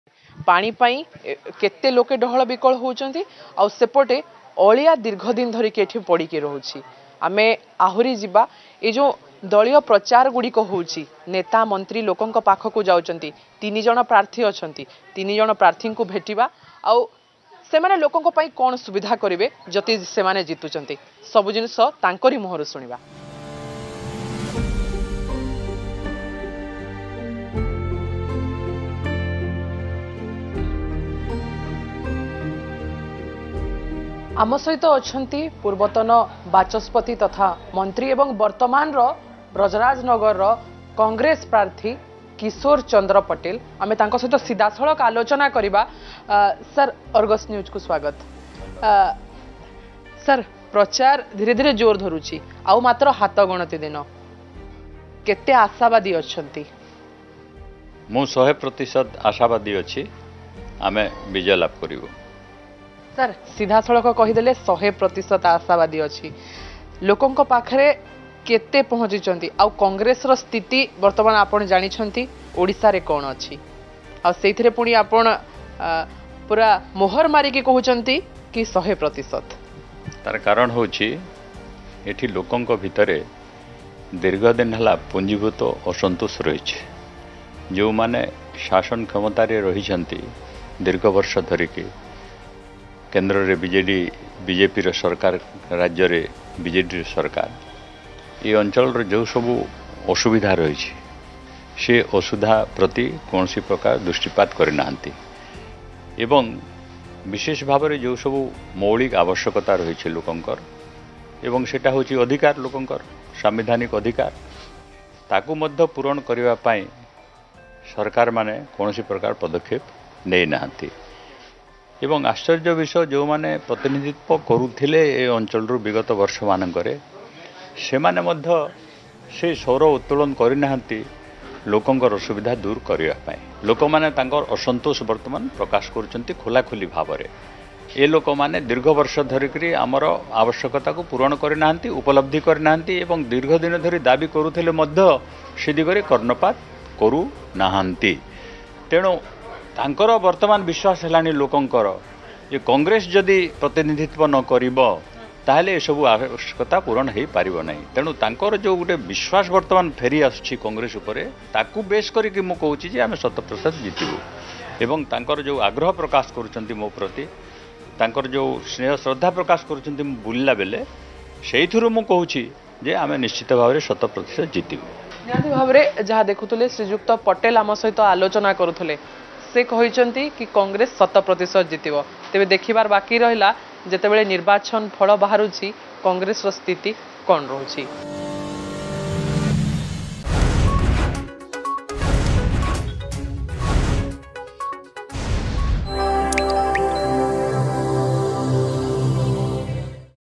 ଜନ ଅସନ୍ତୋଷ ଉପରେ କଂଗ୍ରେସର ଭରସା
ବିଗତ ଦିନରେ ବ୍ରଜରାଜନଗରର ପ୍ରତିନିଧିତ୍ବ କରିଥିବା ନେତା ଏଠିକାର ଅସୁବିଧା ଦୂର କରିବା ପାଇଁ ସ୍ବର ଉତ୍ତୋଳନ କରି ନାହାଁନ୍ତି । ଜନ ସାଧାରଣଙ୍କ ଭିତରେ ଅଛି ପୁଞ୍ଜିଭୂତ ଅସନ୍ତୋଷ । ଏହାକୁ ଅସ୍ତ୍ର କରି ଉପନିର୍ବାଚନରେ ଜିତିବେ ବୋଲି ଆଶା ରଖିଛନ୍ତି କଂଗ୍ରେସ ପ୍ରାର୍ଥୀ କିଶୋର ପଟେଲ ।